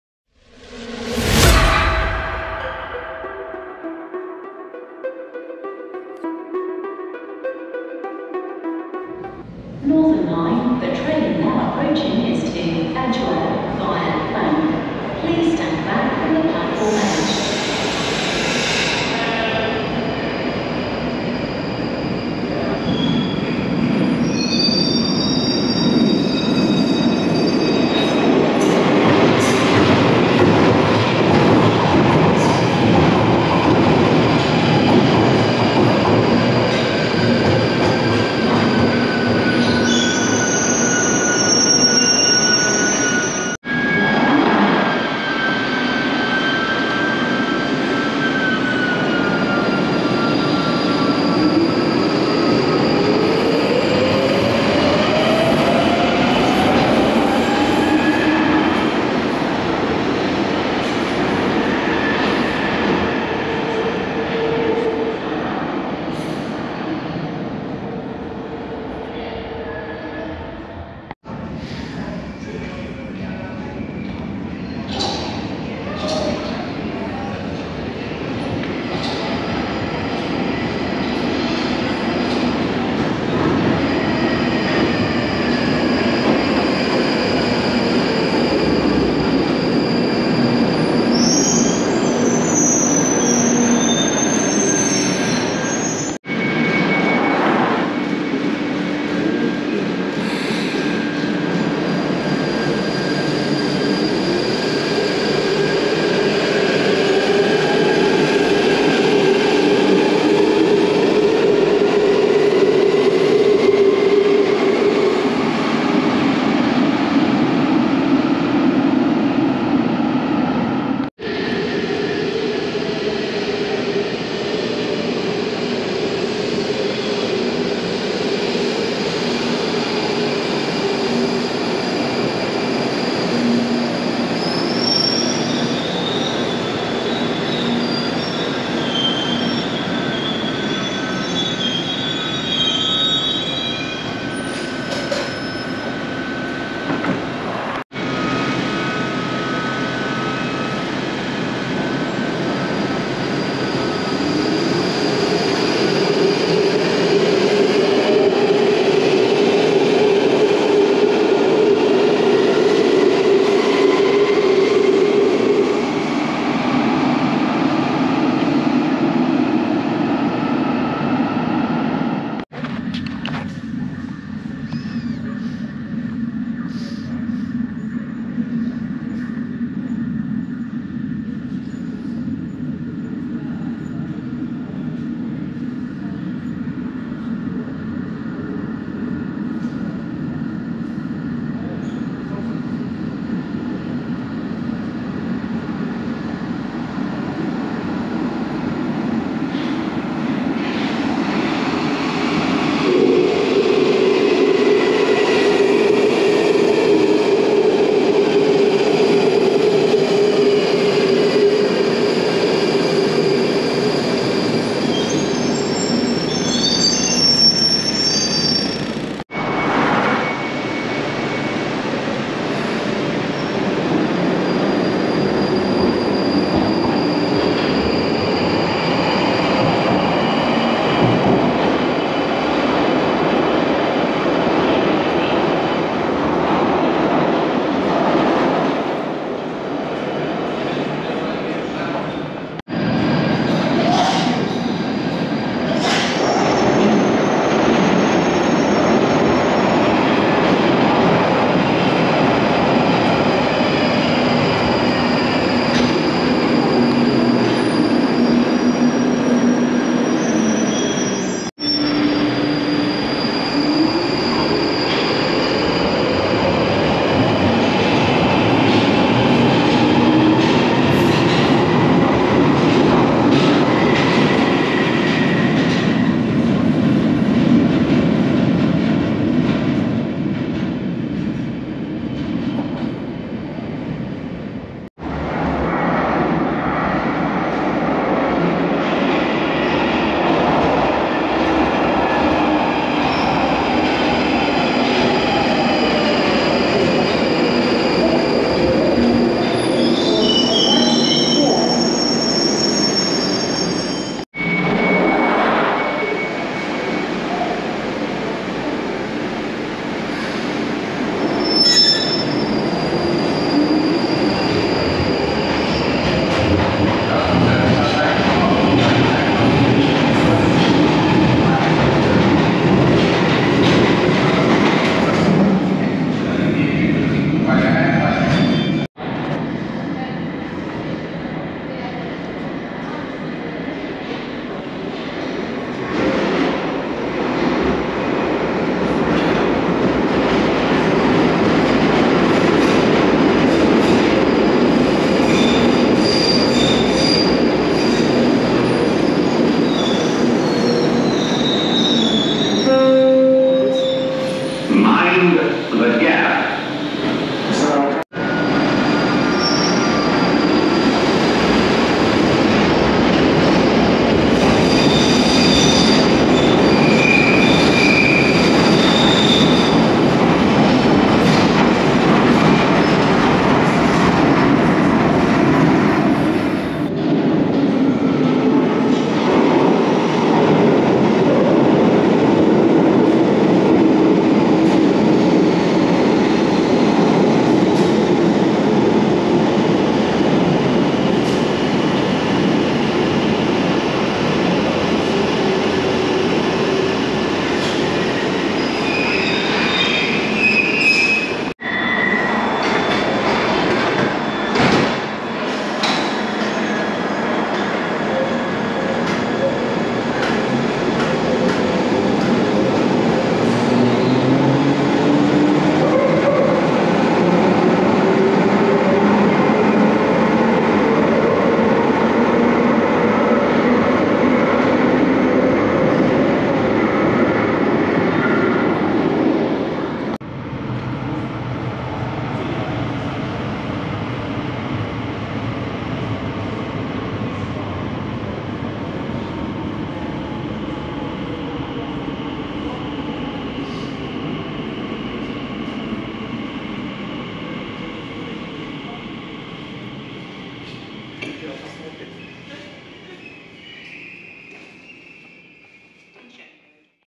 London Underground Observations 25th June 2023
This is some bonus footage on the London Underground I wanted to share filmed on the 25th June 2023

Lines:
Bakerloo Line
Northern Line

Stations:
Balham
Kennington
Nine Elms
Charing Cross
Embankment
Baker Street